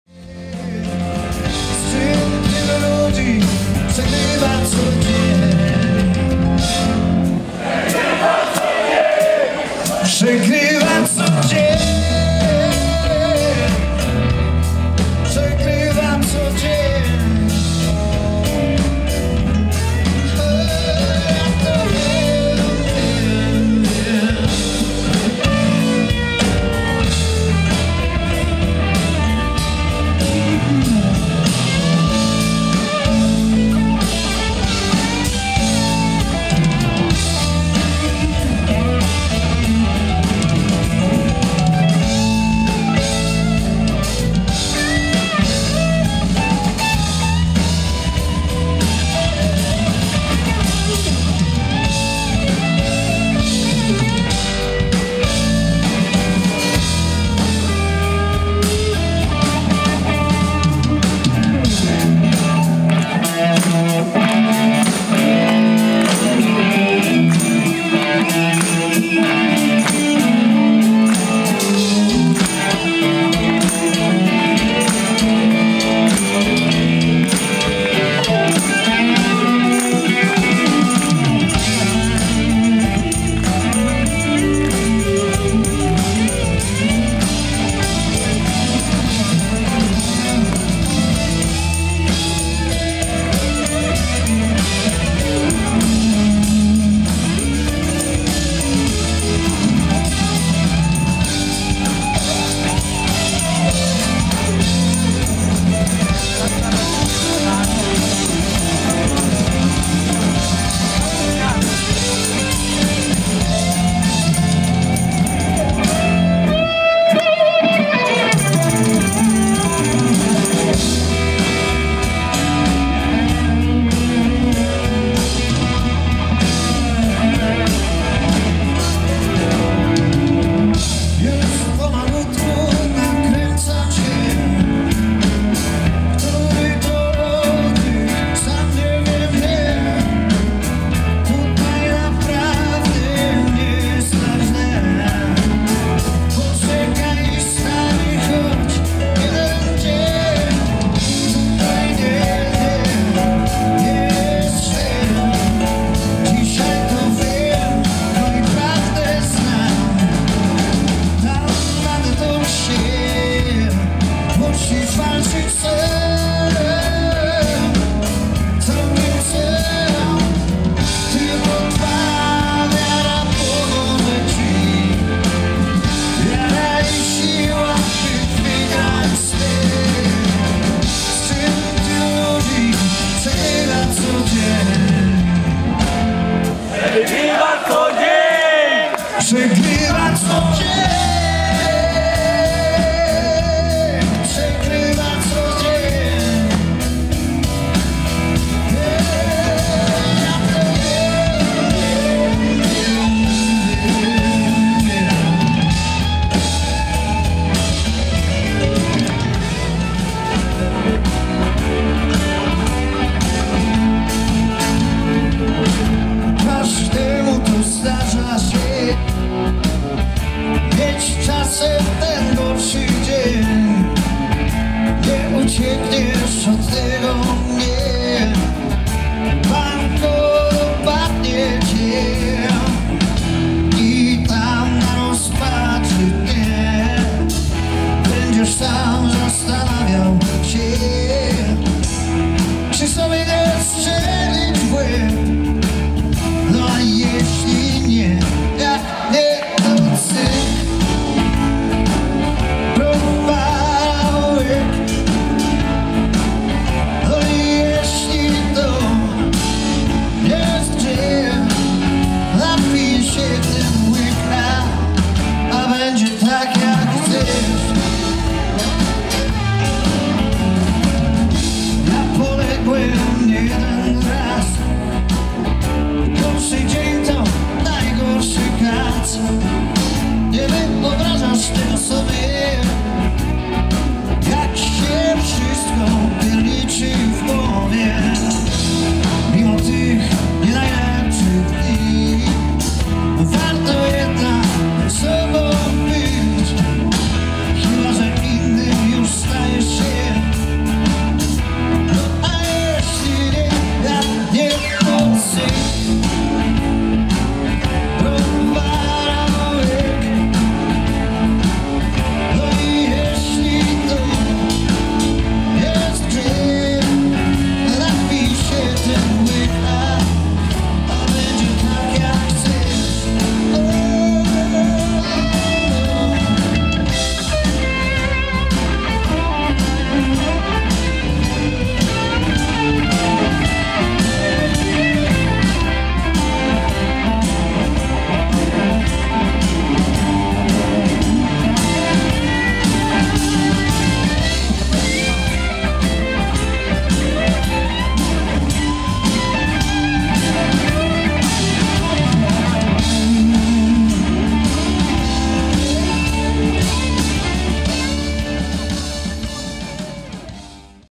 OLSZTYN24: Występ Dżemu na Olsztyńskich Nocach Bluesowych
Fragmenty występu Dżemu na Olsztyńskich Nocach Bluesowych | Amfiteatr im. Czesława Niemena w Olsztynie | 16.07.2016